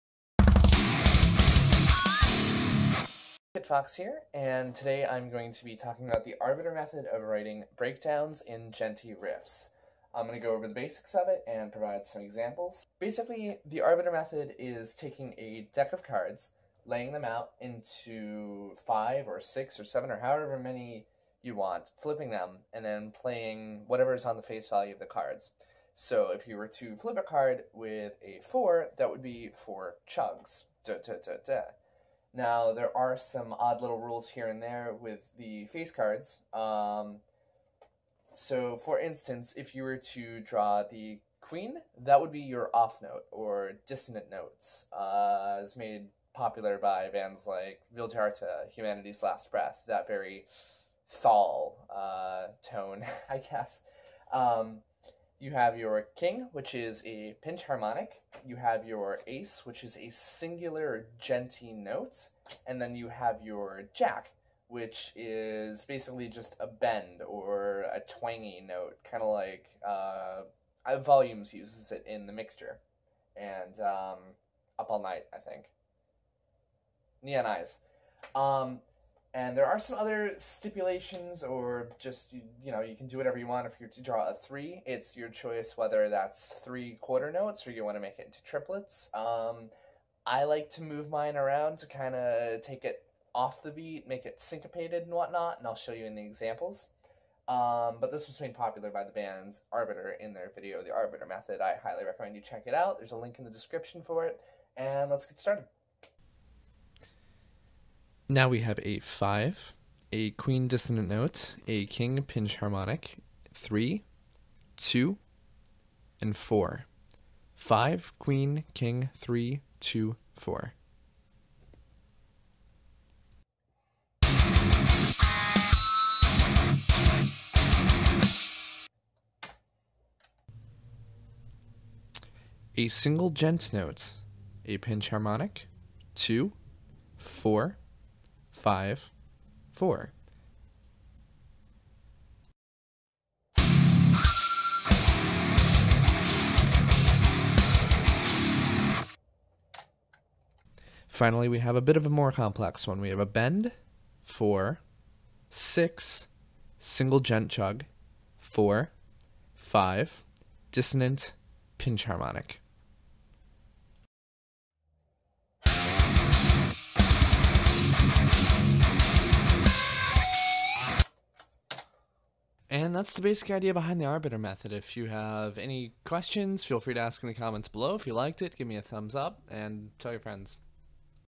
The Arbiter Method Breakdowns
A shitty video where I explain my use of an odd method to create breakdowns, and djenty riffs.